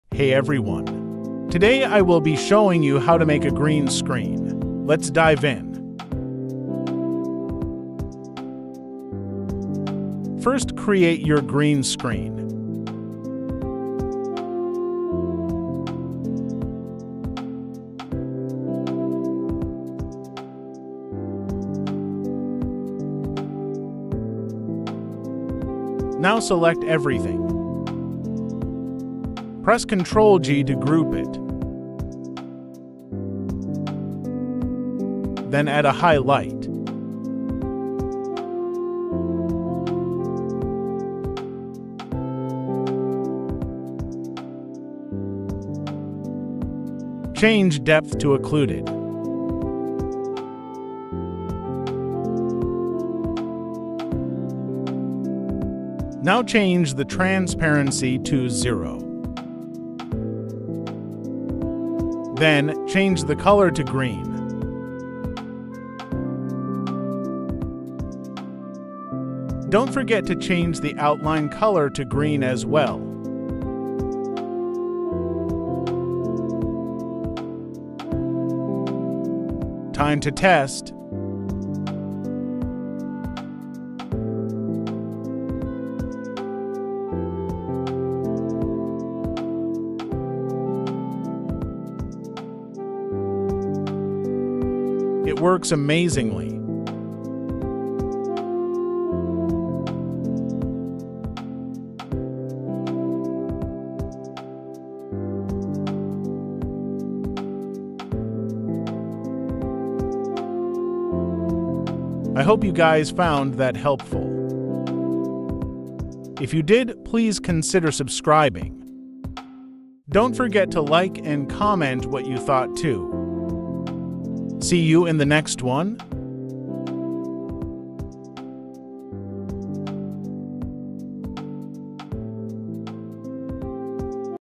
How to make a GREEN SCREEN | Tutorial #3 | Roblox Studio
Hey Everyone! Welcome to my third roblox studio tutorial. I hope you guys found this video helpful! Lots of people have been struggling to render builds. Instead they go the long way and use blender. This way is quick and easy! I hope it helped! :D

(I used this method in my lasts video's thumbnail (How to make a highlight)

0:00 - 0:10 - Introduction
0:11 - 1:50 - Tutorial
1:51 - 2:15 Outro